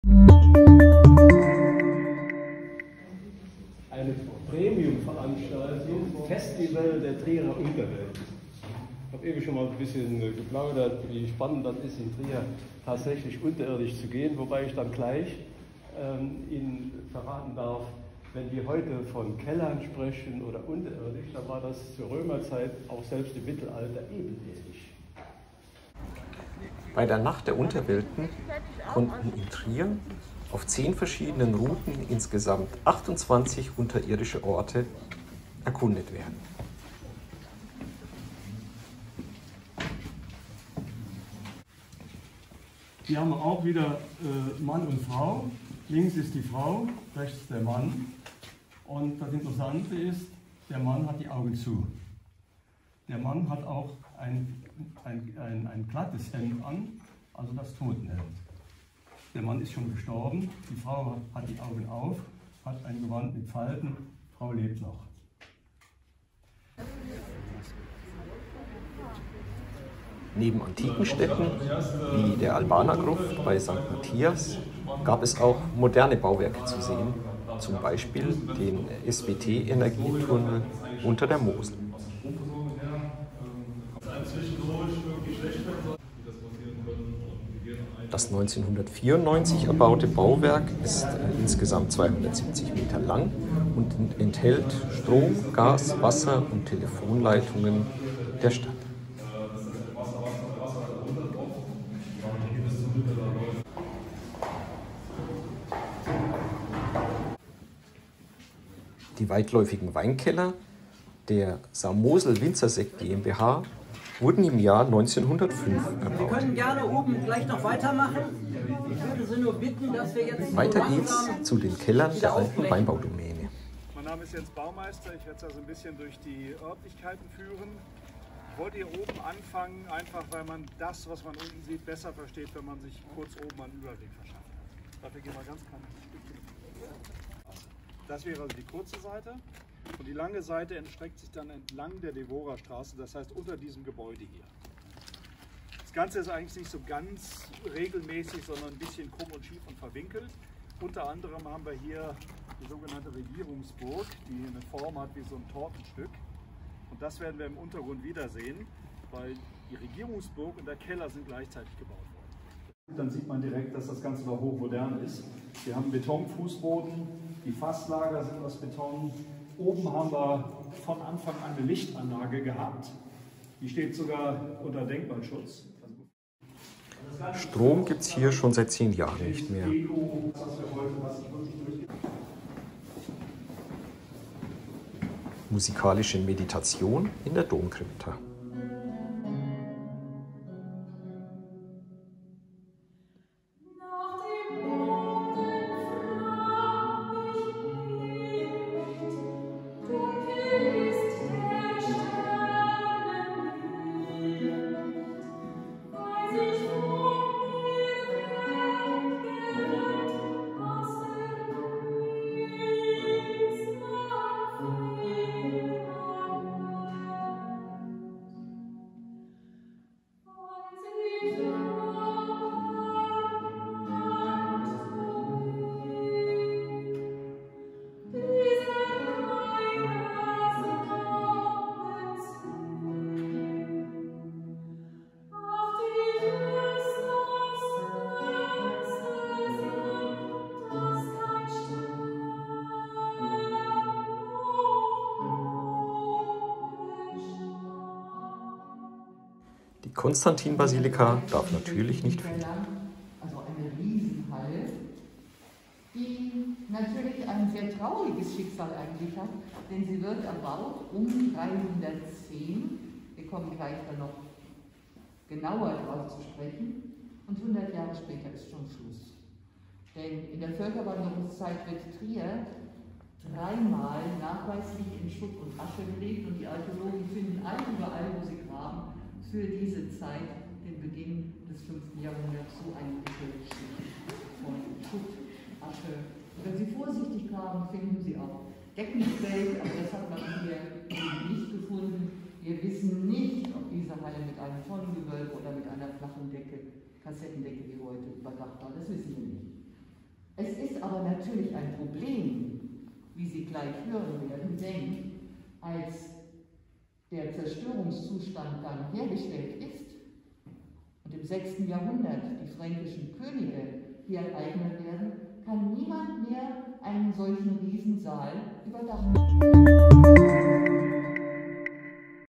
Nacht der Unterwelten in Trier
Unterwegs zu mystischen und spannenden Orten in Triers Unterwelt